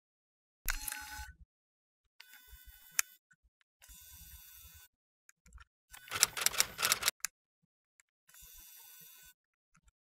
Design New intro Video Gimp with Hitfilm Express/motion graphic/effect sound
In this video you will show intro video I have been designed in hitfilm express with gimp 2.10 i hope you like it friends